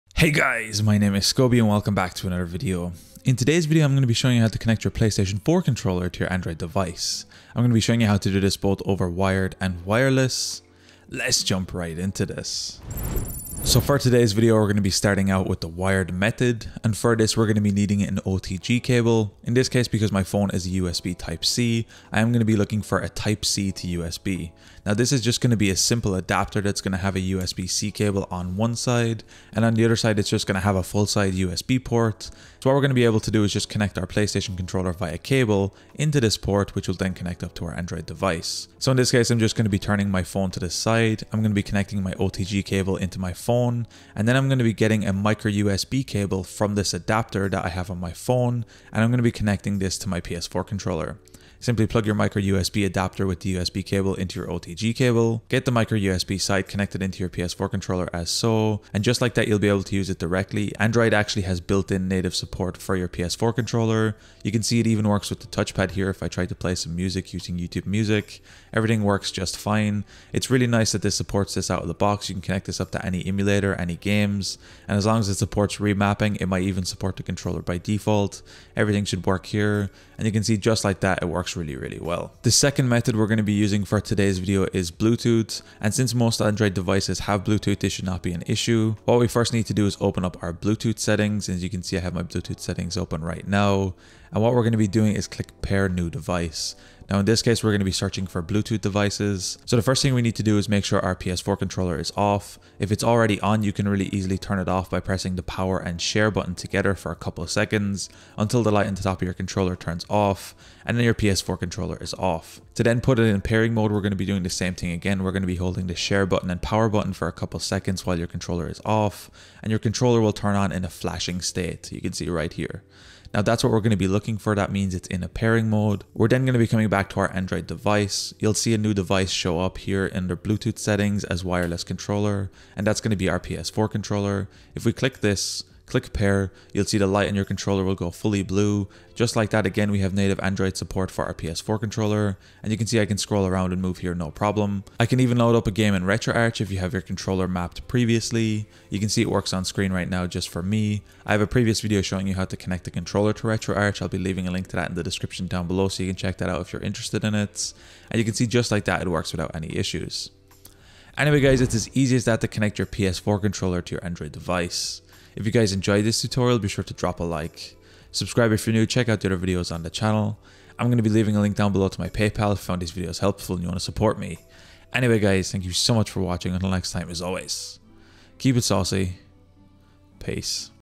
How to Connect Your PS4 Controller to Android! PS4 Controller with Wire or Wirelessly to Android
Hey, guys, my name is Scoby Tech and in today's video, I am going to be showing you how you can connect a PS4 controller to your android device!

This is going to be a nice quick and easy tutorial where I show you all the ways you can connect your PS4 controller to your Android Device!

PS4 Controller:
Amazon US
Amazon UK

Micro B Cable:
Amazon US
Amazon UK

OTG Cable:
Amazon US
Amazon UK